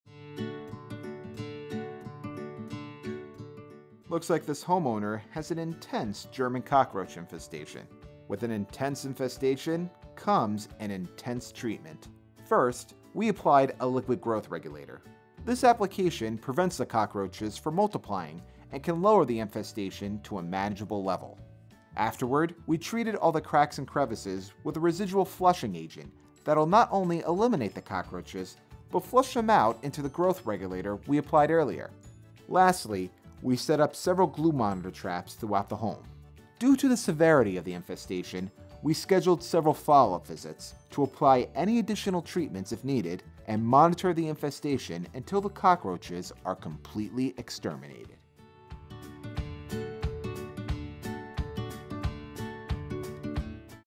A Severe Cockroach Infestation Needs an Intense Treatment in Brick, NJ
This was quite a severe German cockroach infestation.